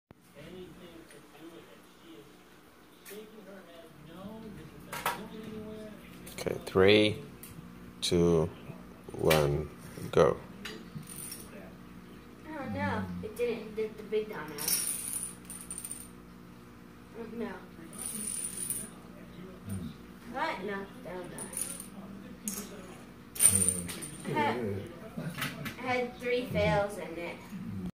Solomon's Mini Dominoes Short Falldown
I am using mini dominoes. They are one forth the size of regular dominoes. I got  these at Bulk Dominies. com. They are Multicolored and Fuchsia. At the end I topple a rainbow tower. There are 3 fails.